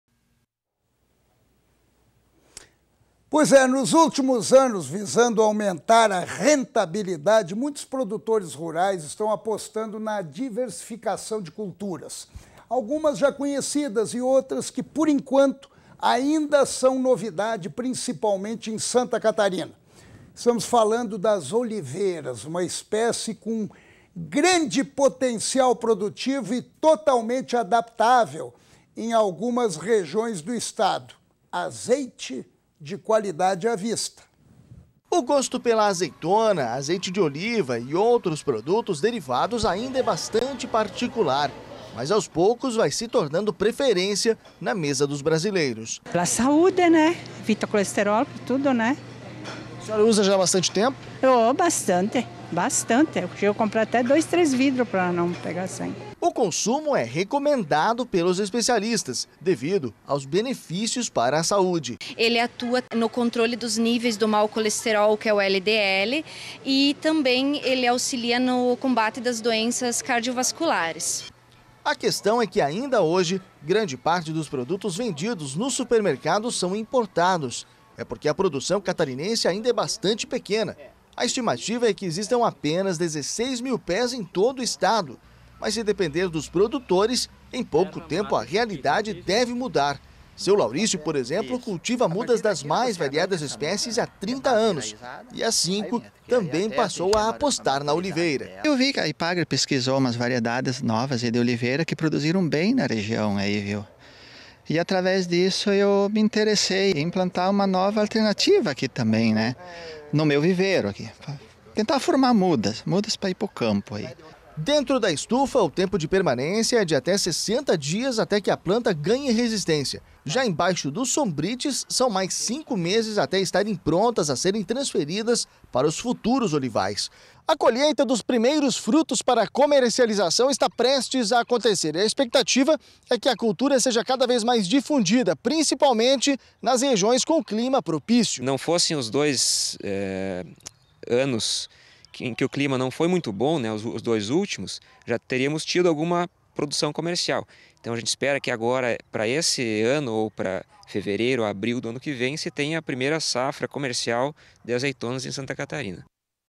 Cresce o cultivo de oliveiras em Santa Catarina
Nos últimos anos, visando aumentar a rentabilidade, muitos produtores rurais estão apostando na diversificação de culturas. Algumas já conhecidas, e outras que por enquanto ainda são novidade, principalmente em Santa Catarina. Estamos falando das Oliveiras, uma espécie com grande potencial produtivo e totalmente adaptáveis em algumas regiões do estado.